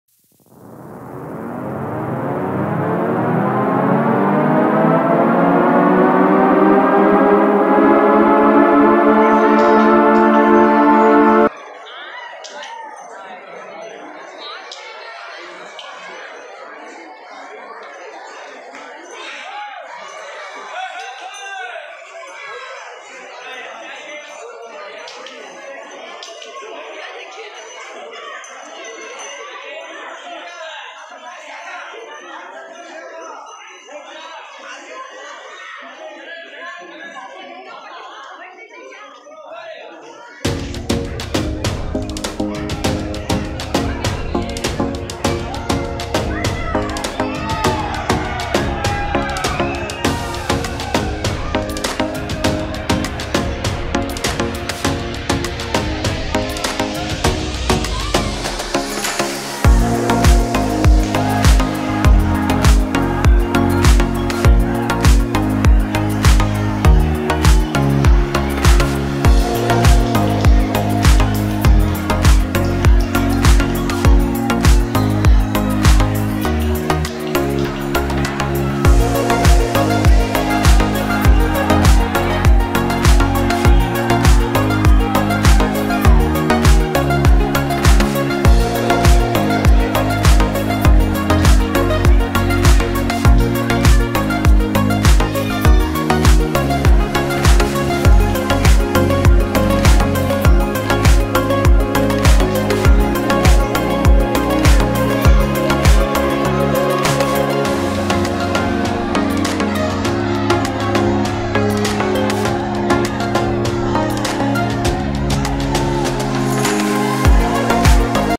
ಮುದಗಲ್ ಪಟ್ಟಣದಲ್ಲಿ ಮೊಹರಂ ದೇವರಗಳ ಸಂಭ್ರಮದ ಸವಾರಿ
ಮುದಗಲ್​ ಮೊಹರಂ
ಮುದಗಲ್​ ಕೊಟೆ
ಹಸೇನ್​ ಹುಸೆನ್ ಸವಾರಿ
ಮುದಗಲ್​ ನ್ಯೂಸ್
ಮುದಗಲ್​ ಹುಸೇನ್ ಬಾಷಾ ಸವಾರಿ
mohuram​ moharam song
raichur​ mudgal moharam